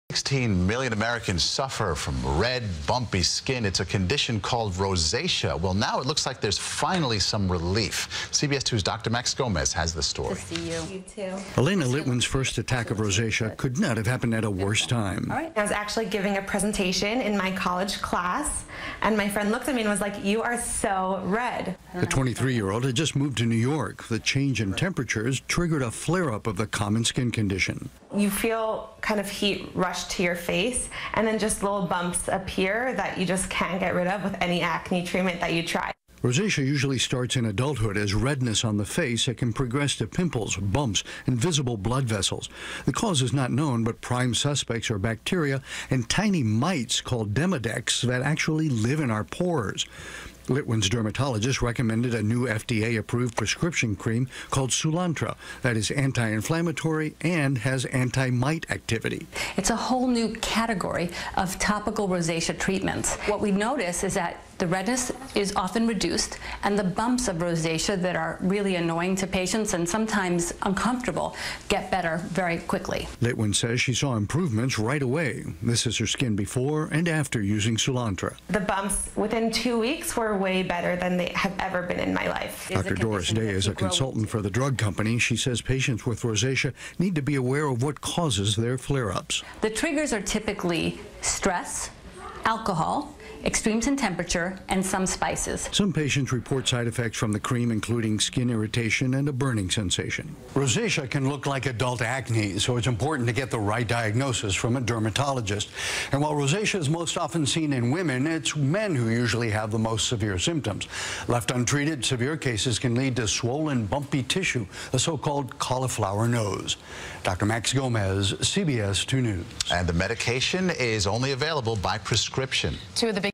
Dr. Max Gomez: Rosacea Treatment
About 16-million Americans suffer from Rosacea, now there may be some relief. CBS2's Dr. Max Gomez explains.